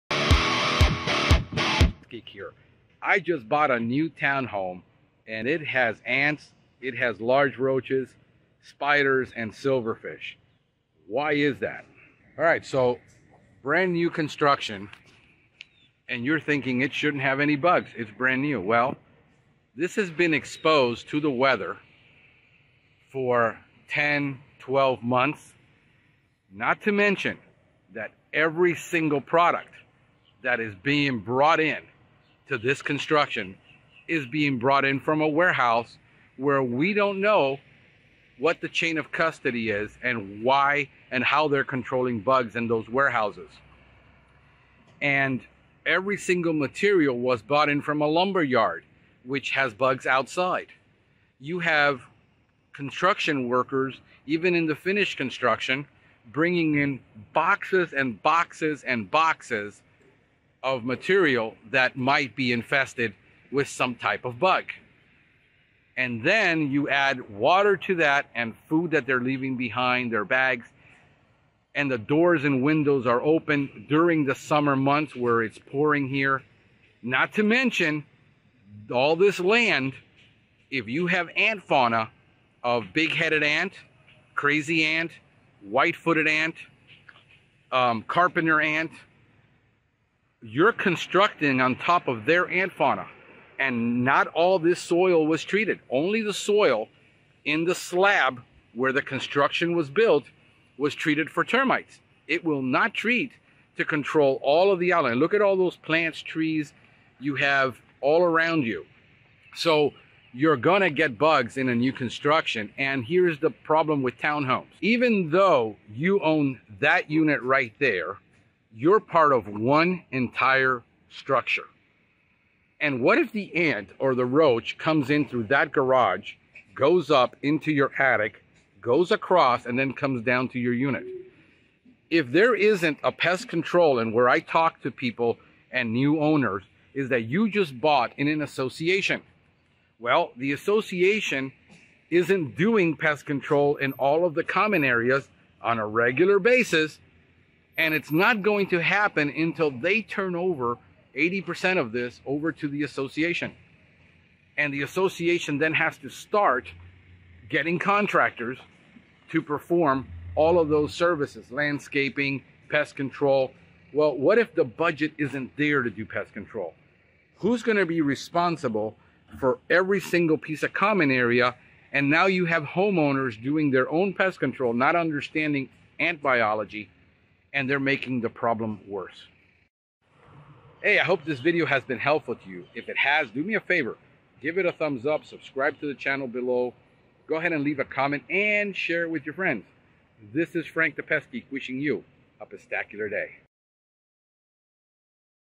I Just Bought a New Townhome In Homestead Florida And it Has Ants, Roaches and Silverfish.
On today's edition of the Pestgeek Podcast, host and integrated pest management expert Frank 'The Pestgeek' Hernandez explains how a beautiful brand new townhouse in Homestead Fl can already be plagued by pests. This brand-new construction can already have ants, large roaches, spiders, and silverfish. So how does this happen? First of all, it has been exposed to the weather for at least 12 months. More importantly, every product brought into this construction comes from warehouses where the chain of custody is unknown. In other words, we do not know if and how pests are being controlled at those warehouses. Material has been brought in from a lumber yard where there are insects. In fact, even in the completed construction, there are boxes and boxes that may be already infested with bugs. To make matters worse, there is food and water left behind. We can also see the land with untreated soil that may have ant fauna such as big-headed ants, crazy ants, white-footed ants, and carpenter ants. Townhomes are a problem because you are one part of a whole structure.